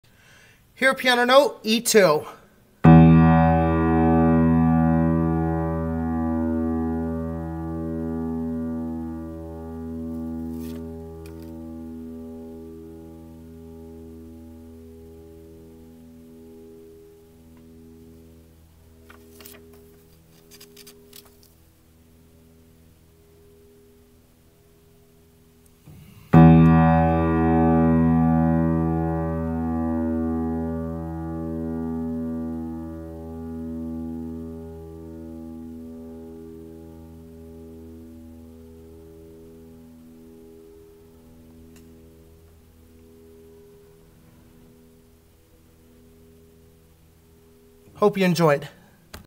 Hear Piano Note E2 on a 1957 Everett Console Piano.
Sounds good, especially after Tuning.